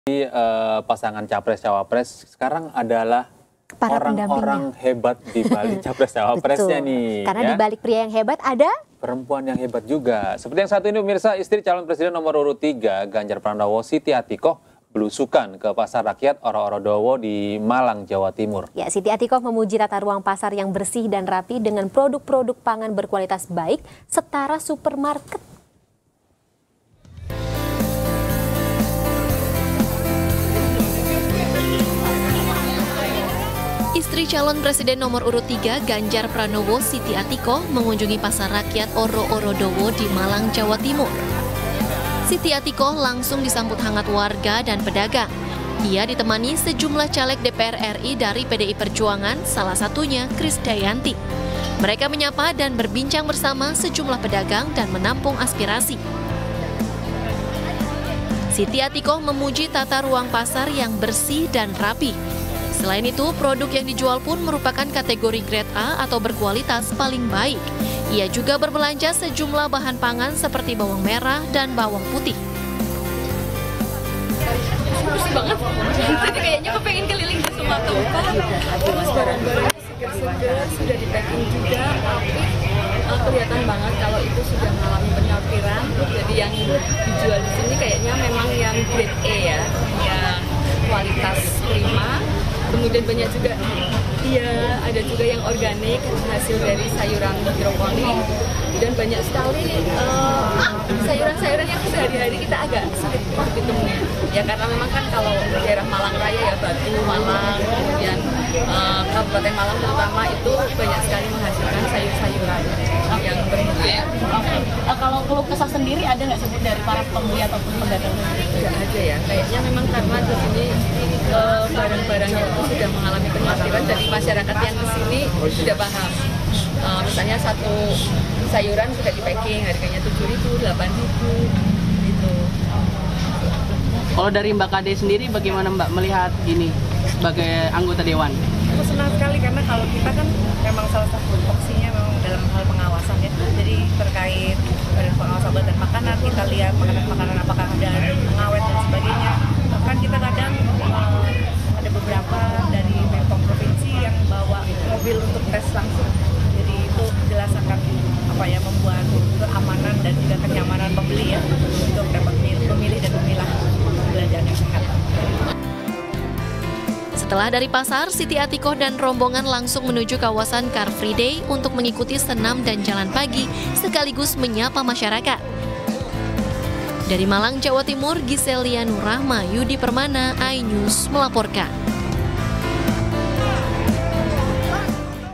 Warga Sambut Siti Atikoh di Pasar Oro-Oro Dowo, Malang
Siti Atikoh Supriyanti istri Calon Presiden Nomor Urut 03 Ganjar Pranowo blusukan ke Pasar Rakyat Oro-Oro Dowo Malang, Jawa Timur, Minggu (28/1/2024). Atikoh terlihat menggunakan kaus bertuliskan Tas Tes yang dipadukan dengan jilbab berwarna pink.